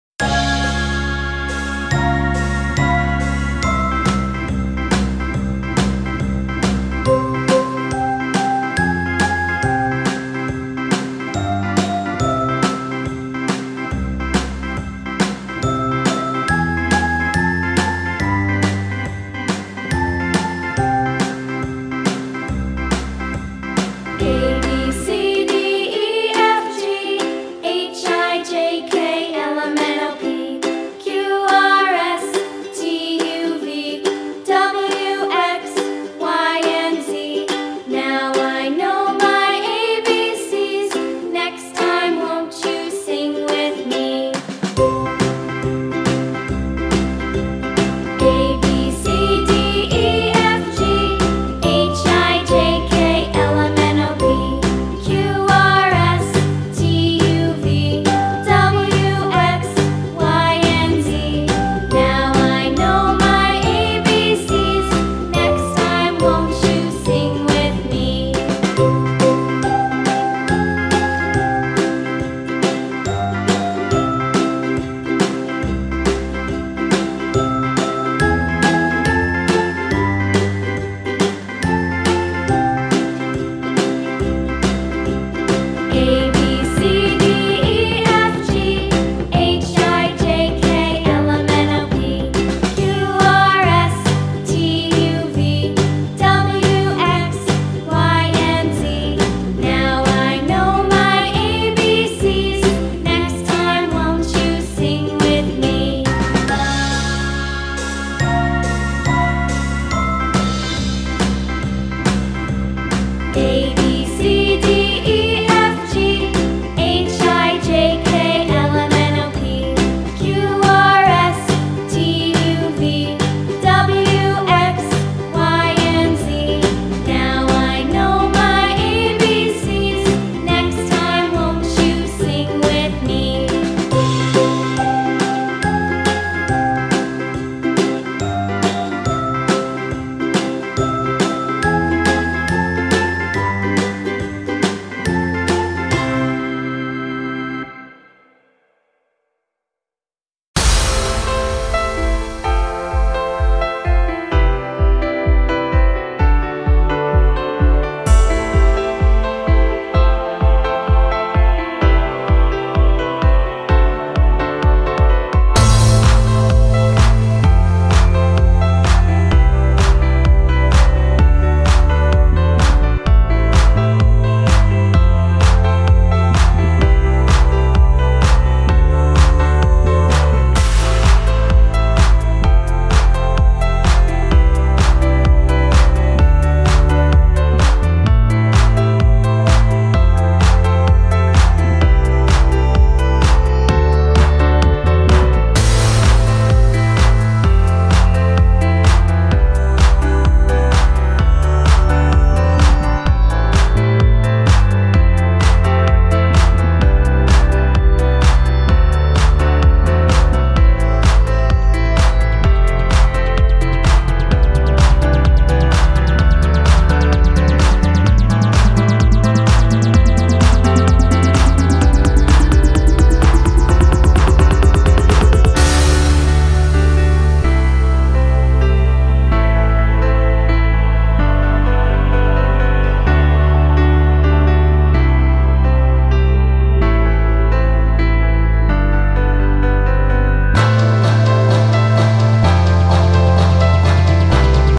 Learn Tomato Drawing and Coloring for Kids Toddlers - How to Draw Tomato Easy Step by Step Kidzaw
Learn Cute Tomatoes Drawing and Coloring for Kids Toddlers - How to Draw Tomato Step by Step  Kidzaw